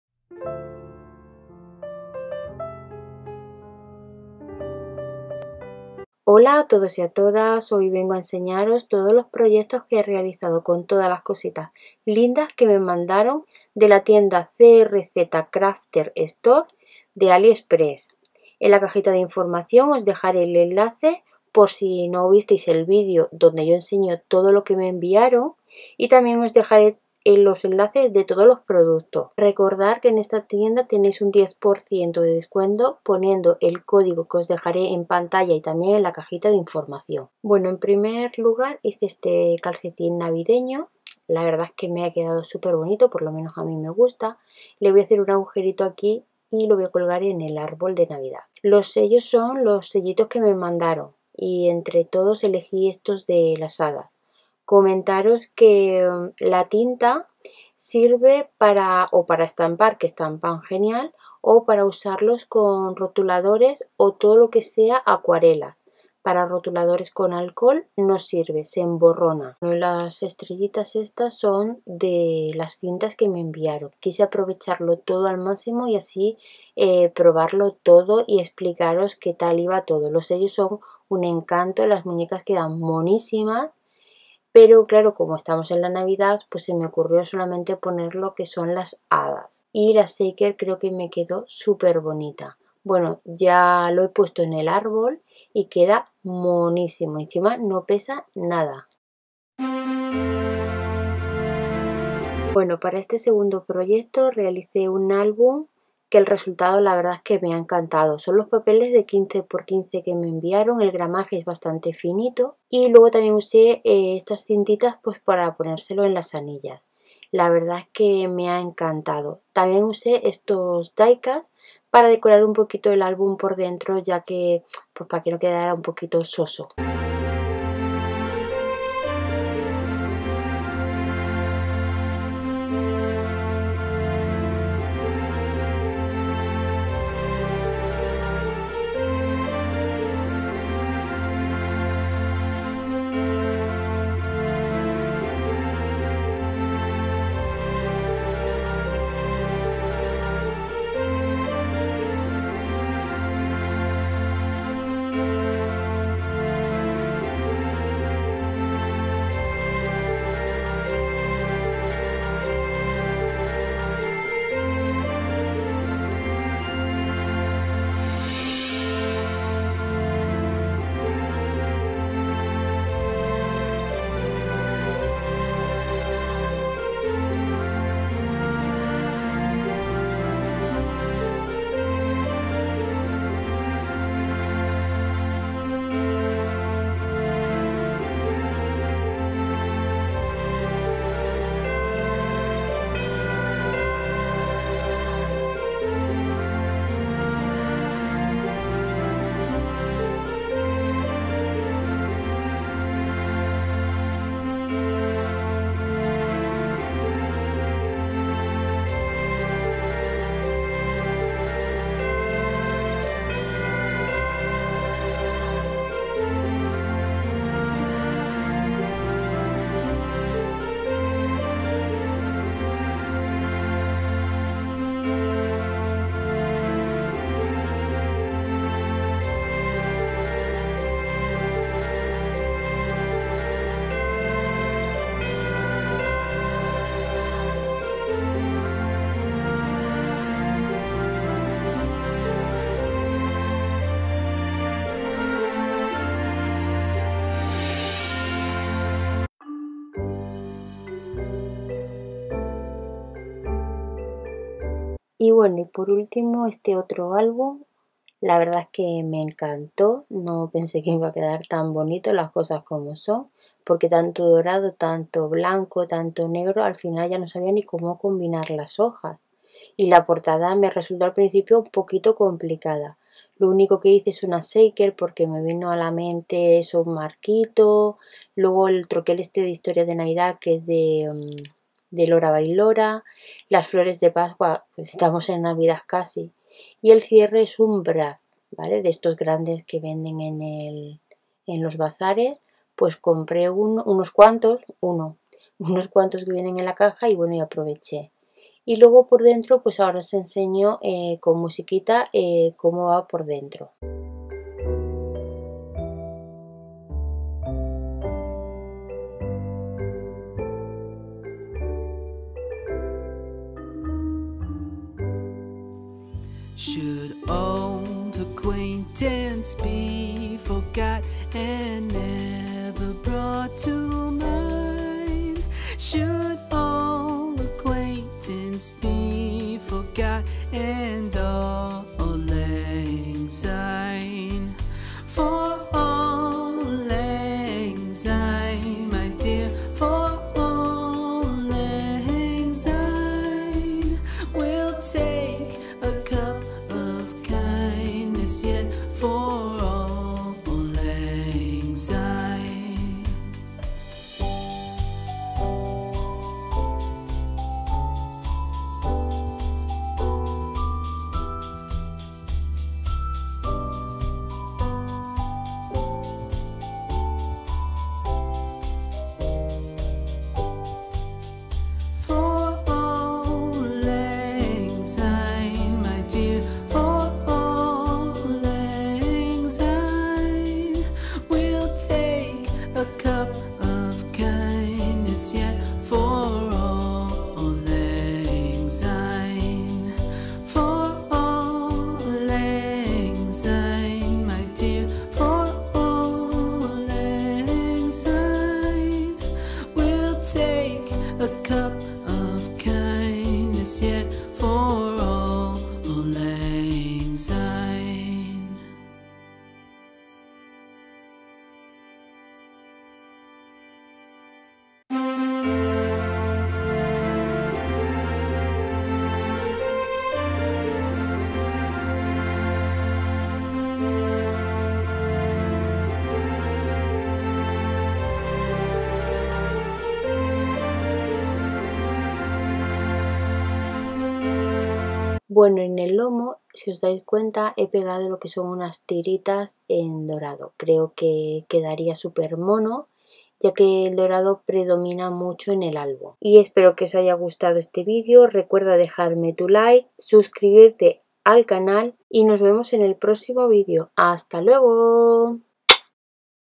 NO TE LOS PUEDES PERDER PROYECTOS BELLOS con la tienda CRZCrafter Store
Hola a todos-as, hoy vengo con los proyectos que realicé con las cositas tan bellas que me mandaron para la  colaboración de la tienda CRZCrafter Store de aliexpress.. Suscríbete a mi canal si aún no lo estás.. y ayudame a que  mi canal siga creciendo y para ello necesito que compartas mis vídeos..espero que os guste y que paséis un lindo día..

-CINTAS NAVIDAD
-DIECUTS 124 UNIDADES DE FORMAS DISTINTAS
-PAPELES NAVIDAD 6X6
-SELLOS DE NIÑAS
-PAPELES A4
-ACETATO CON FOIL DORADO
-TINTA NEGRA